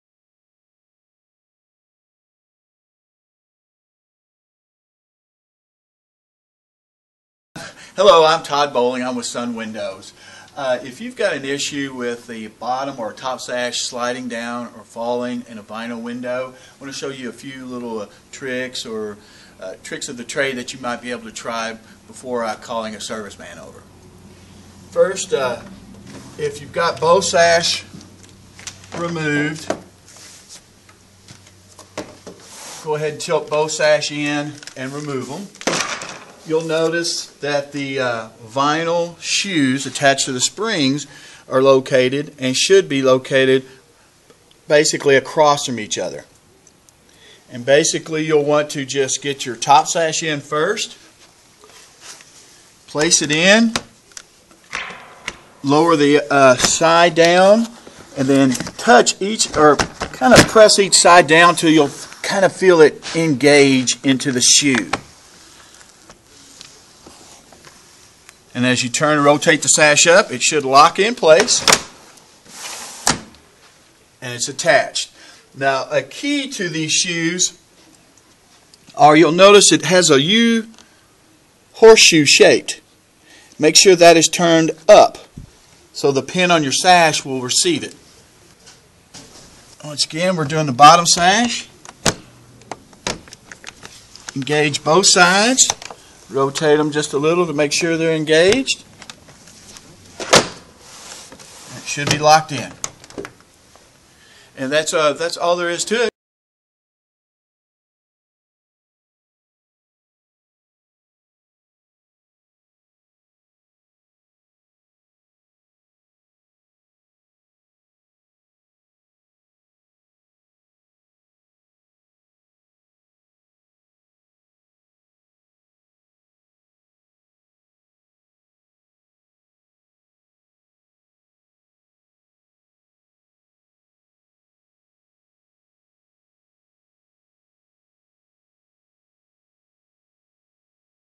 Properly Seating a Sash Pin in a Vinyl Window Shoe
If you are having operating issues with your vinyl windows, watch this video first to see it will fix the issue for you.